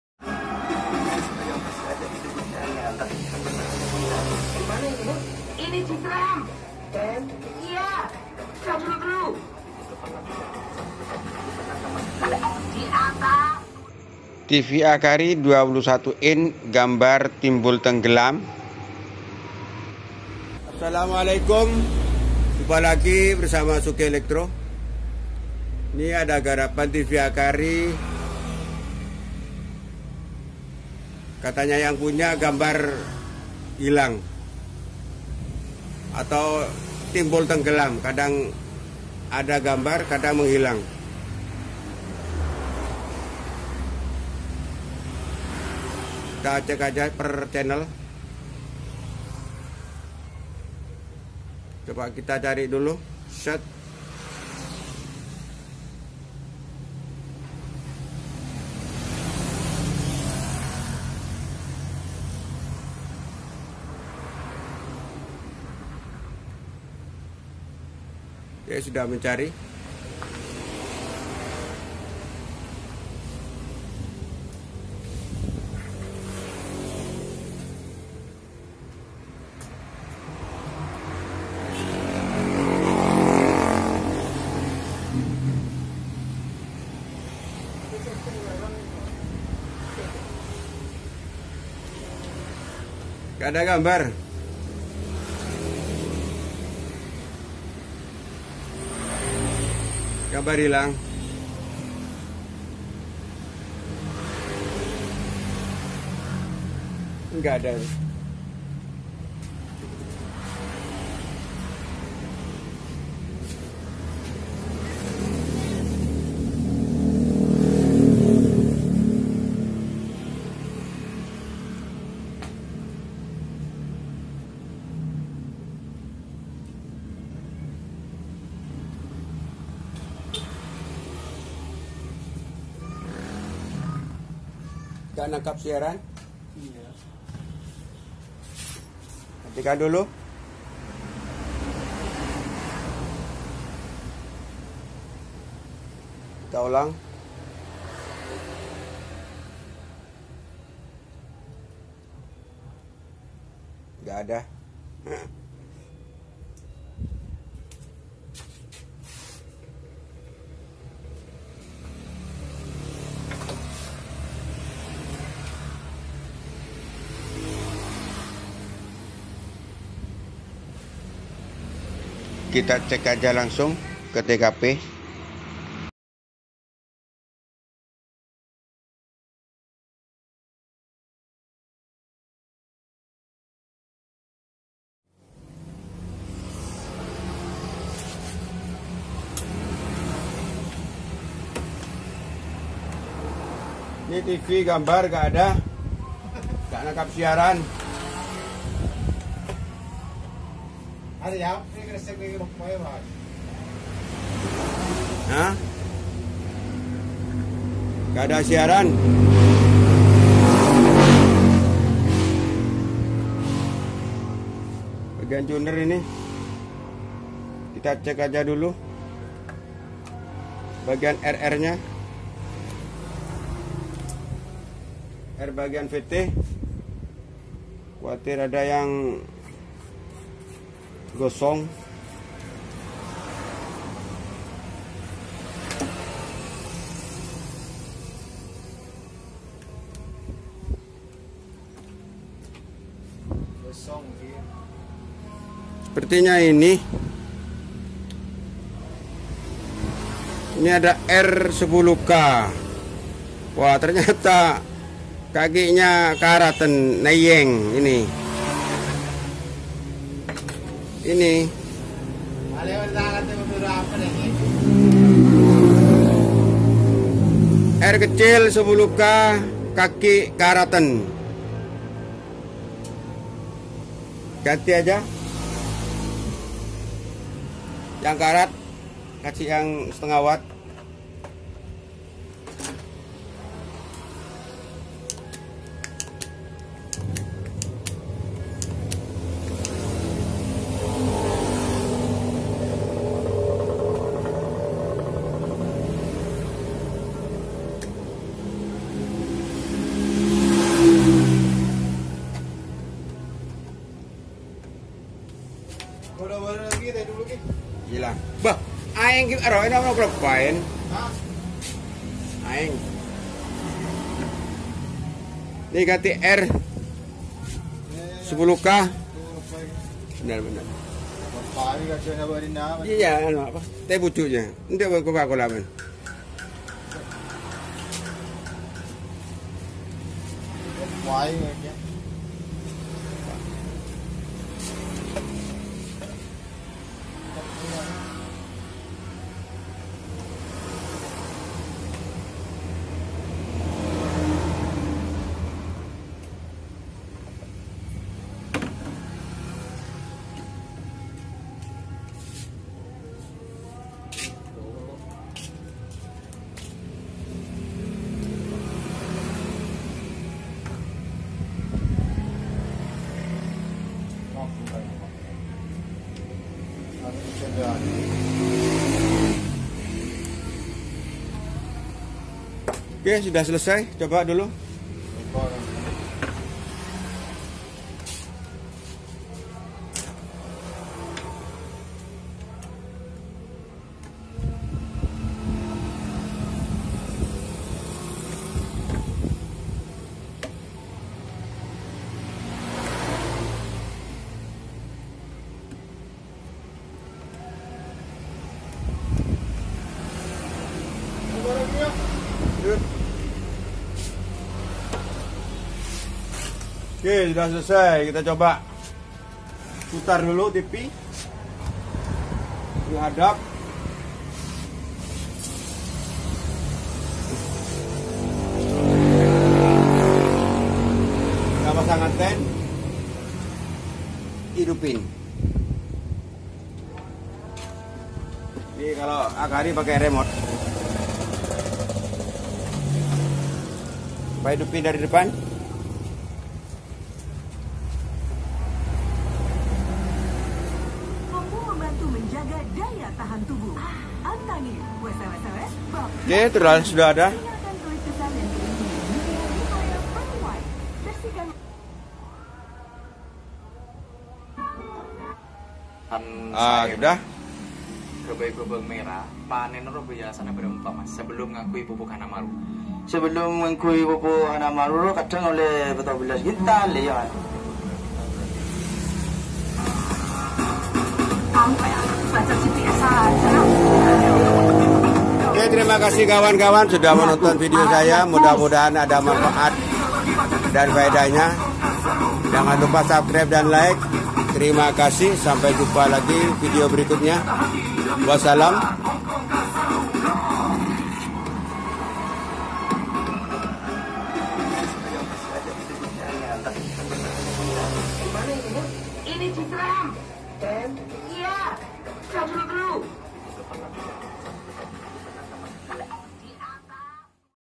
TV AKARI TIDAK ADA SIARAN KADANG ADA GAMBAR KADANG HILANG vlog 024
gejala gambar kadang ada kadang hilang TV Akari tidak nangkap siaran di sebabkan karena tuner sebagai kakinya tidak ada tegangan atau sebagian tegangan hilang di sebabkan karena komponen yang korosi di area VT
dukungan anda untuk memberikan subscrib like commend dan share saya ucapkan terimakasih
@tehnisi tv @bengkel tv @tukang tv @tukang service @service elektronik @update